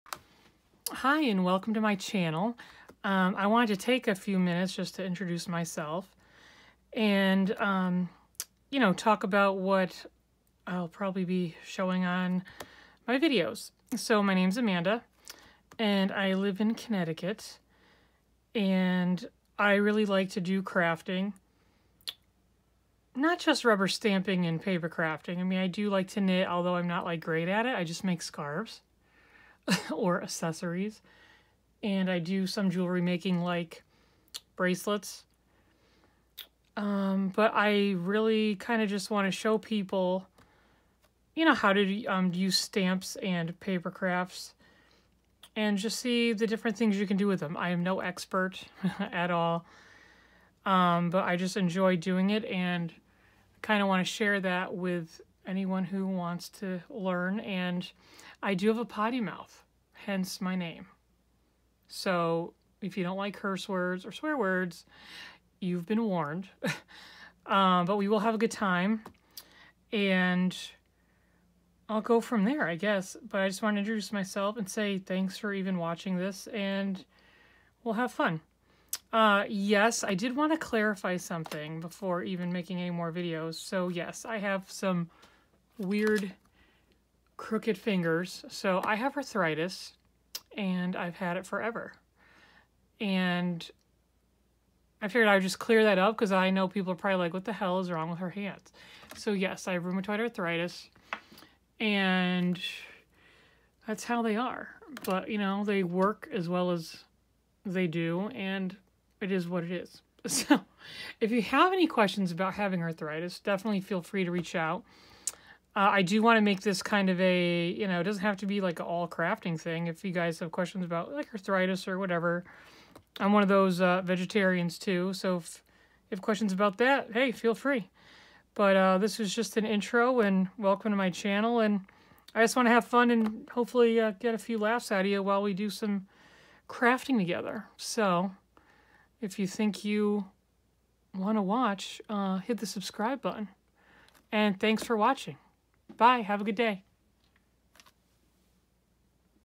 Allow me to introduce myself!
Thanks so much for stopping by.  In this video I introduce myself and explain why my hands look so weird.  If you have any questions about arthritis, feel free!  Thanks!